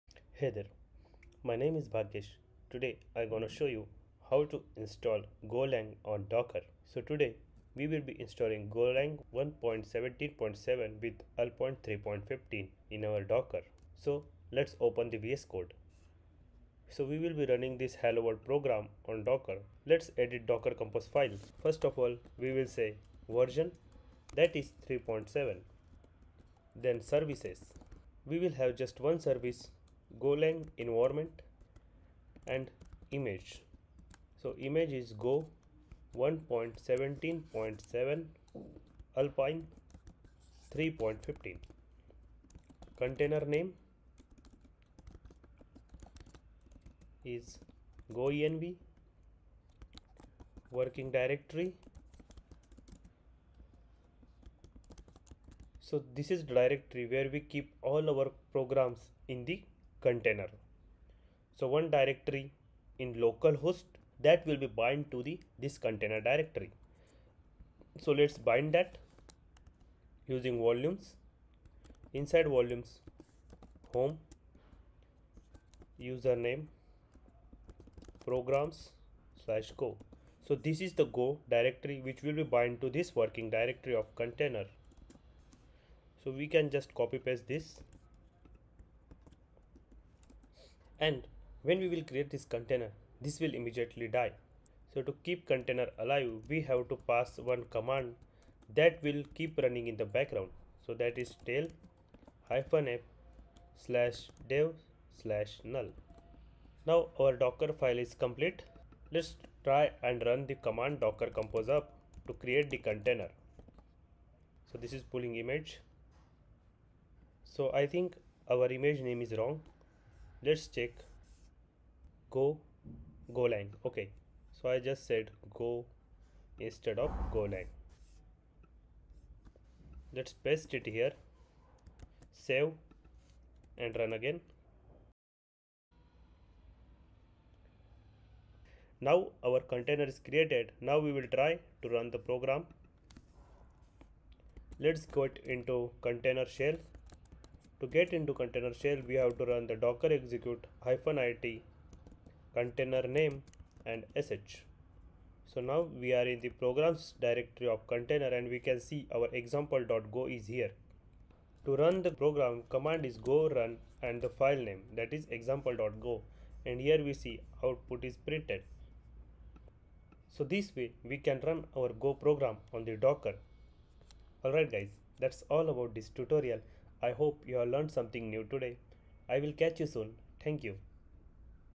Install Latest GoLang on Docker | Run Go Program / Code on Container Environment
We will install GoLang on Docker using Docker Compose file, we will be running Go code on docker environment without installing it in Operating System